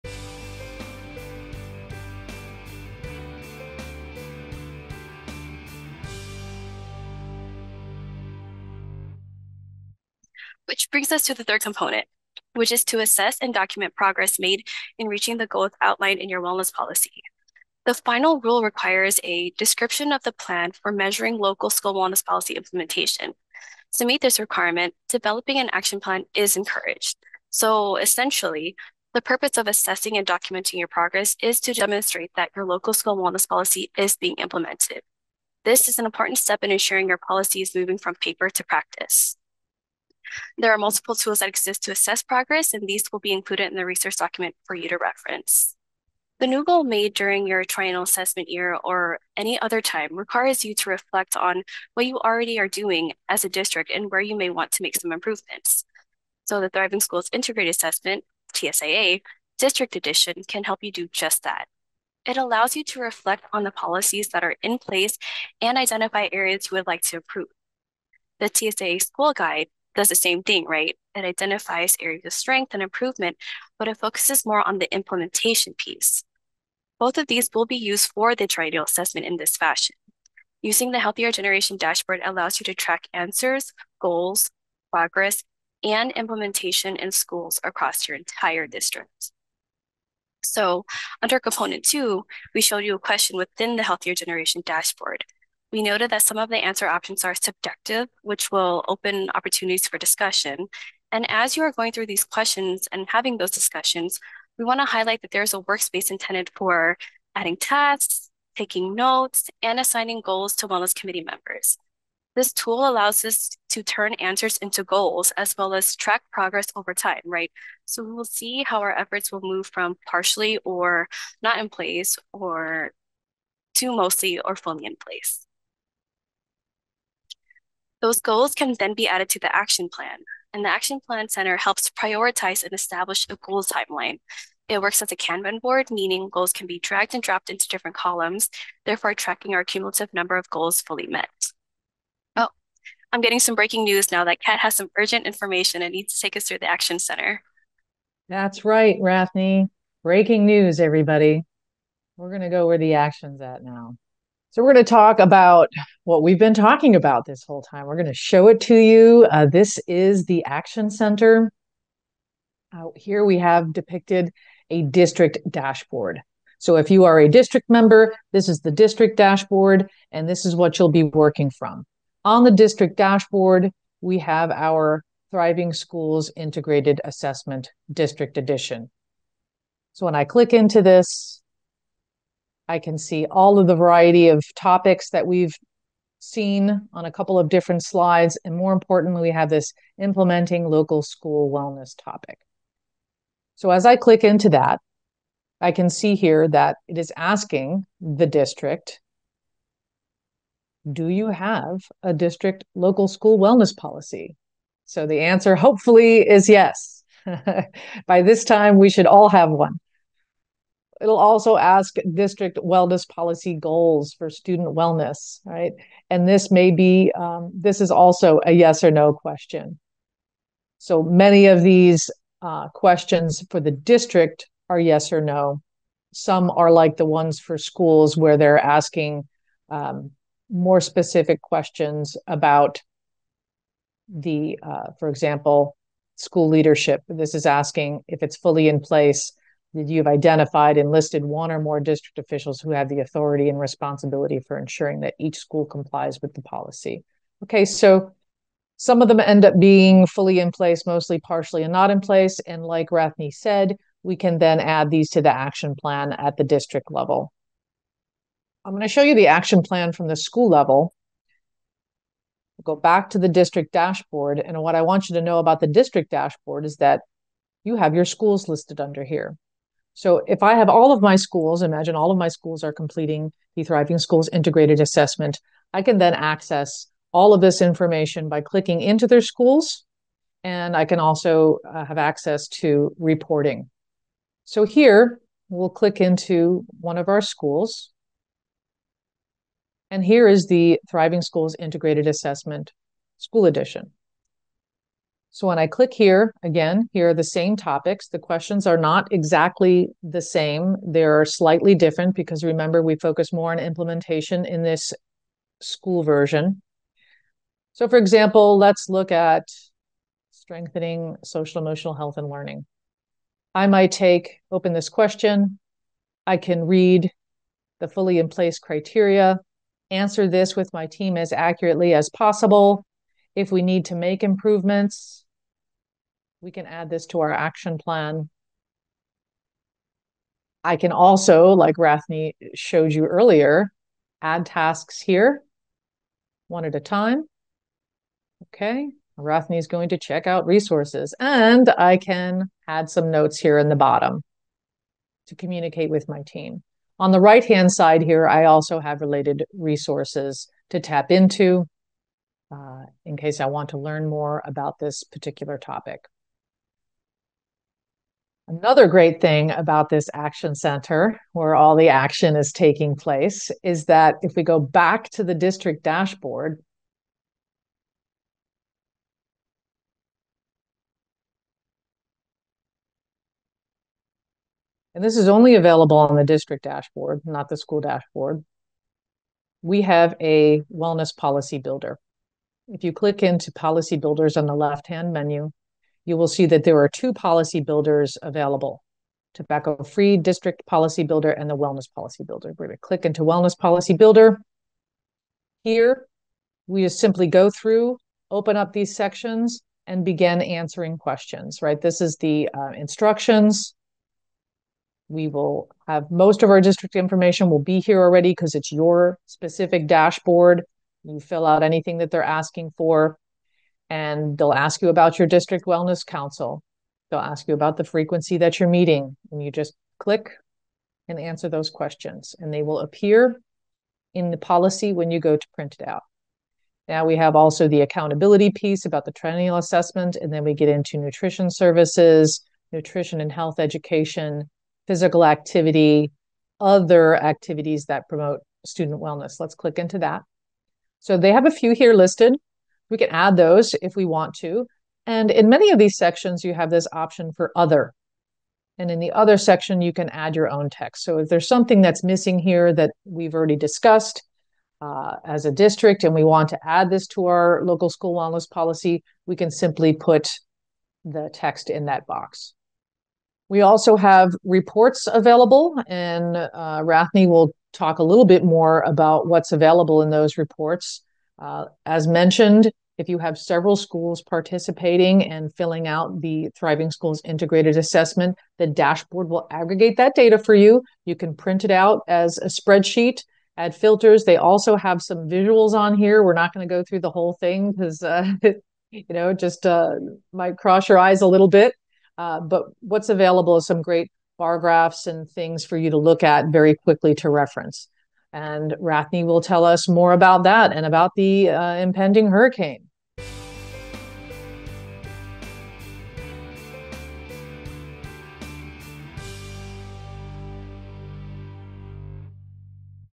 Effectively Communicating your Local School Wellness Policy/Putting it into action. - Video 5of 6
Video #5 of 6: Triennial Assessment Component #3
Speakers: Rathney Min and Kat Satterley
As we strive to strengthen local school wellness policies (LWSP) in all areas of whole child health, we want to ensure LEAs have the tools and resources to implement policies and strengthen best practices.

In this session we will:
-Briefly discuss the required components of a LSWP according to USDA Final Rule
-Make recommendations to strengthen LSWPs in all areas of whole child health
-Introduce the Wellness Policy Builder
-Provide methods for communication and feedback on the LSWP to your school community
-Show how the Thriving Schools Integrated Assessment (TSIA) can be used to identify existing best practices and areas of opportunity.